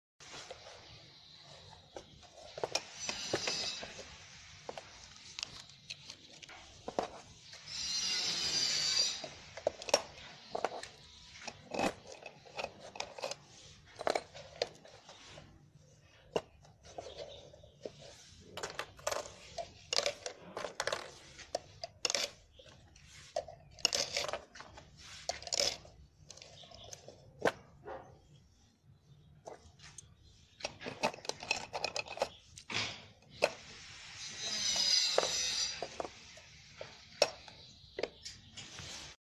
Mini Book Piggy Bank
This Mini Book Piggy Bank is made from strong steel plates covered by high quality cardboard in an English Dictionary cover design. It can easily be concealed on a bookshelf. Perfect for hiding cash and collectibles. Great gift for children!